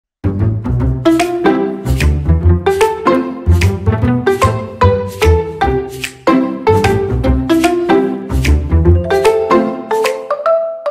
Animal Trivia: What Part Of The Body Do BUTTERFLIES Use To TASTE? Animal Trivia
Did you guess correctly? Share your answer in the comments below!